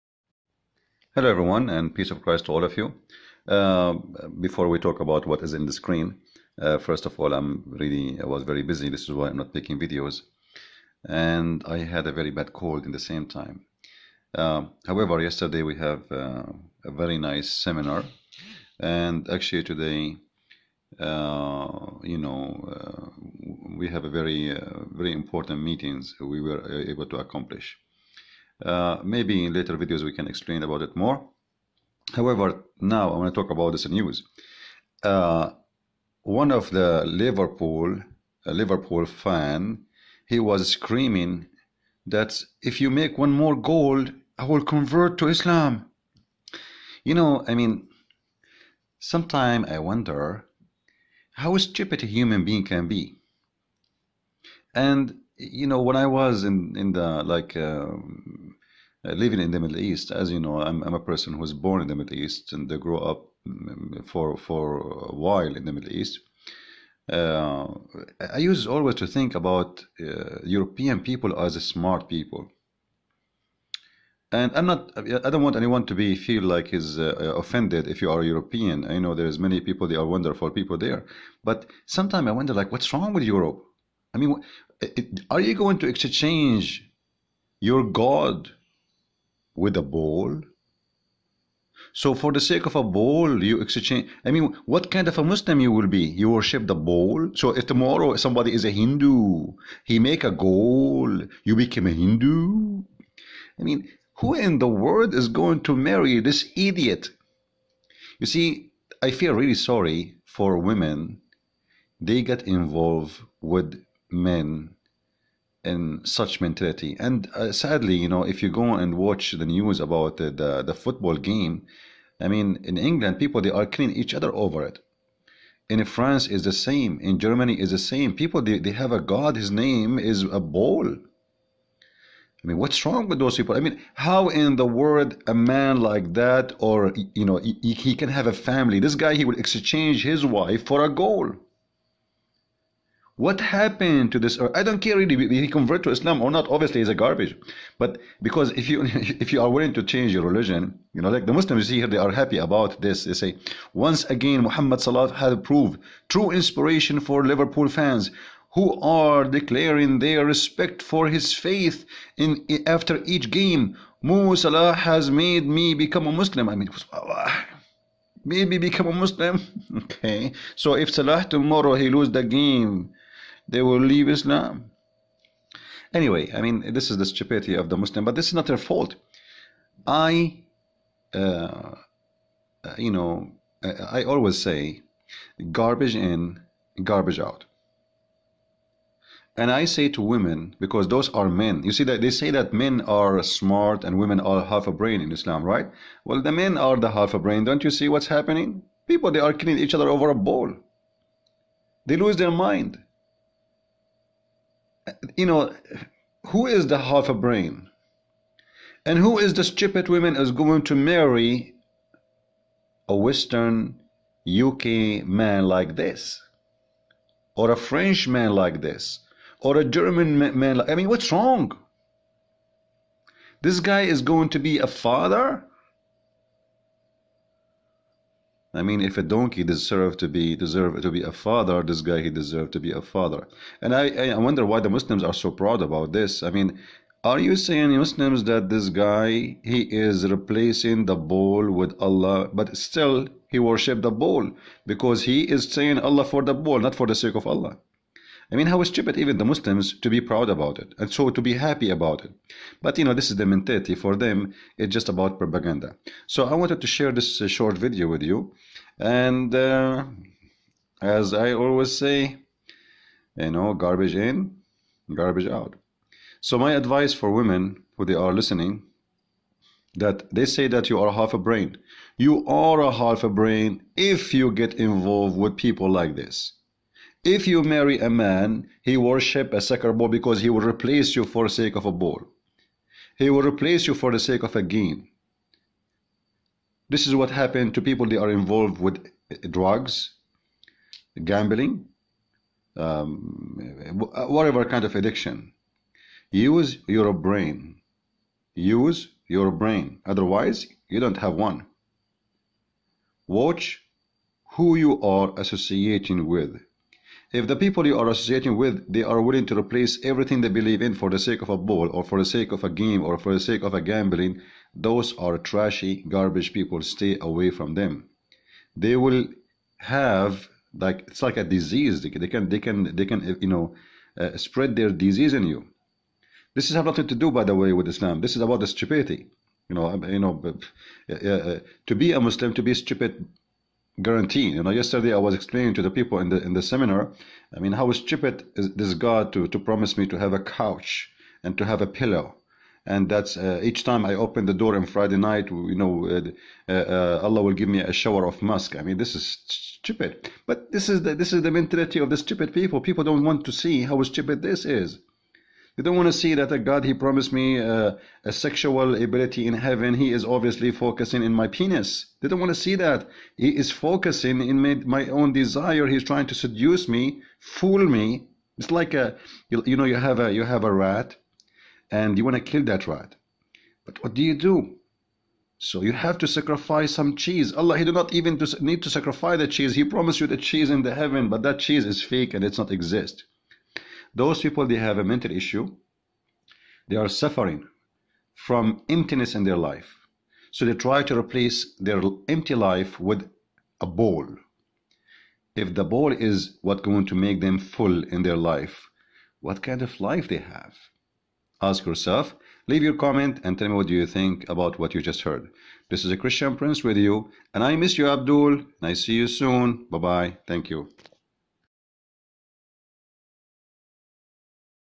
Christian Prince - A Liverpool FC fan Exhanging Allah for a FOOT BALL
LIVERPOOL – A Liverpool FC fan has tweeted that the team's new signing Mohamed Salah is a great player and a great person who would turn him Muslim. “He Gonna (sic) turn me Muslim,” the fan, Carson Coffied, replied in a tweet posted on Liverpool's official account. The tweet was posted to celebrate